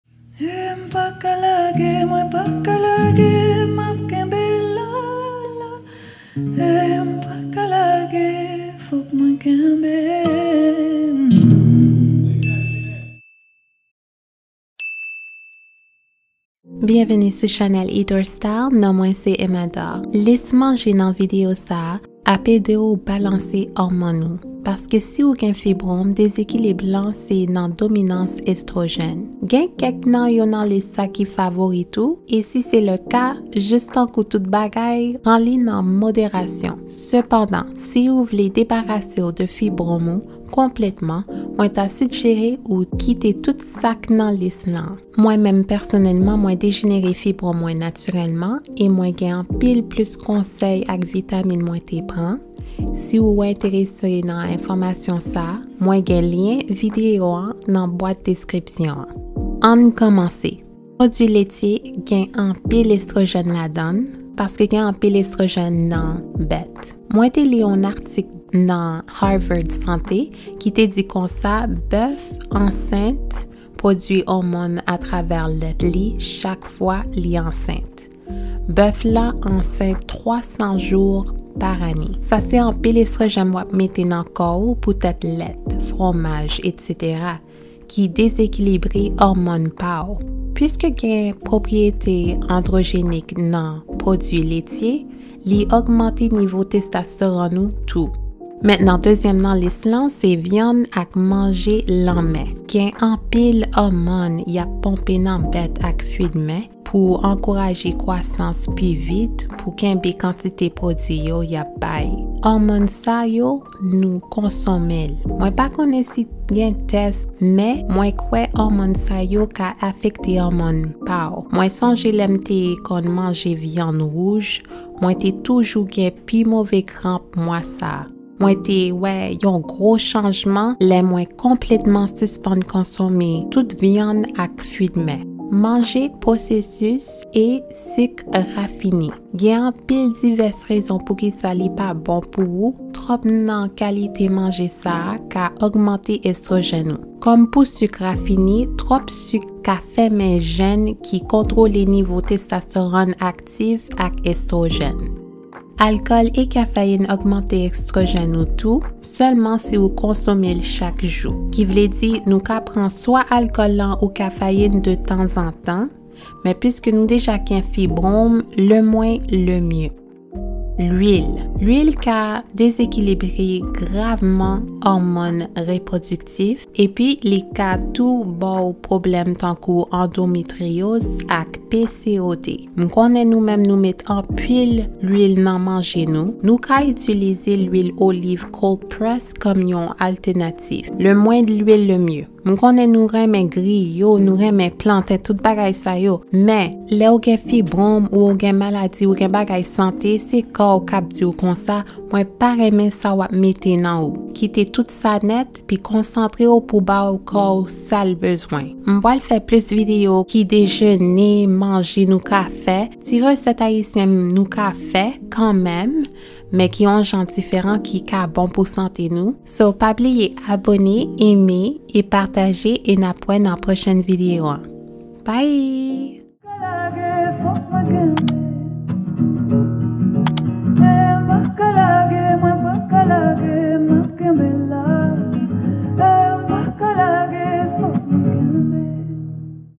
Manje Pou Evite Pou Retresi Oswa Dejenere Fibwòm
Mwen pa yon doktè oswa espesyalis. Enfòmasyon sa a baze; sou rechèch, mwen te fè pou dejenere fibrom mwen yo natirèlman.